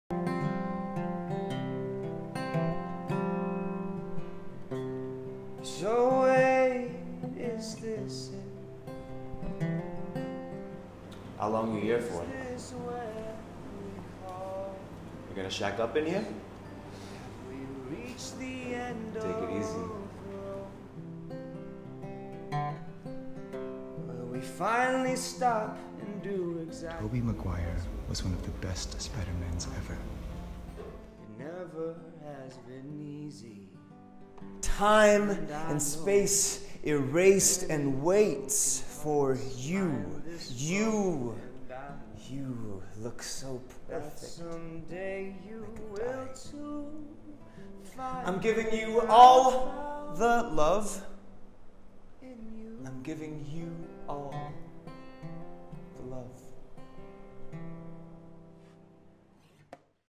New Ways to Love Nathan
Fronting a offbeat disposition, our Feb'19 cover star Nathan Hartono draws you in with his rebellious magnetism as he gives Men’s Folio an exclusive preview of his multifaceted talents.